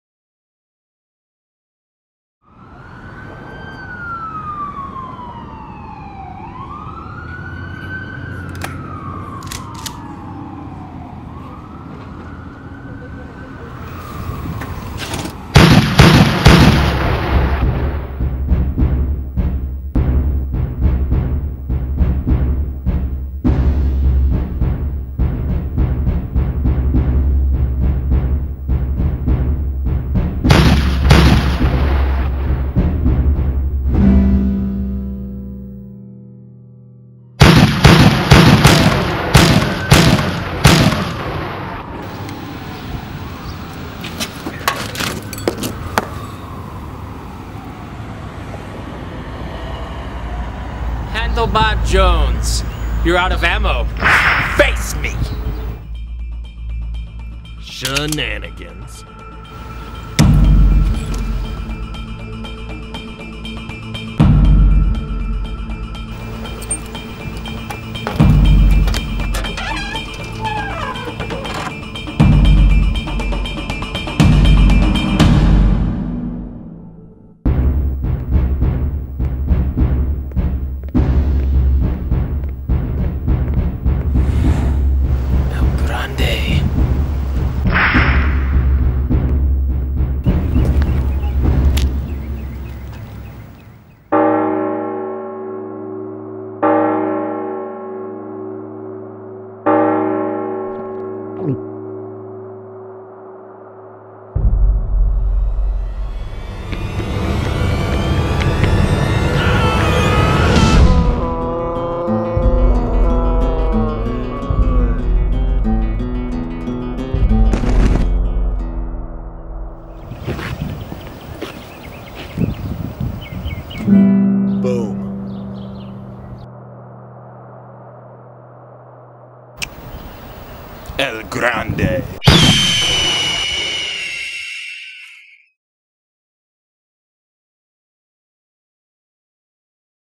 Handlebar Jones: El Grande
Stuck in a shootout with his arch nemesis Accento Incorrecto, Handlebar Jones finds himself out of ammo and out of time.
To survive, our hero must rely on an old friend, "El Grande".